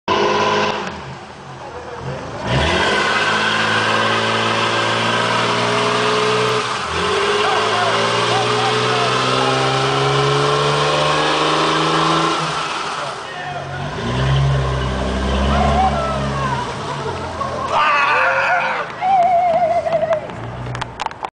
Auto Tech burnout
Juan burning out in his v6 mustang while mr. whitley can't control anything haha, bastard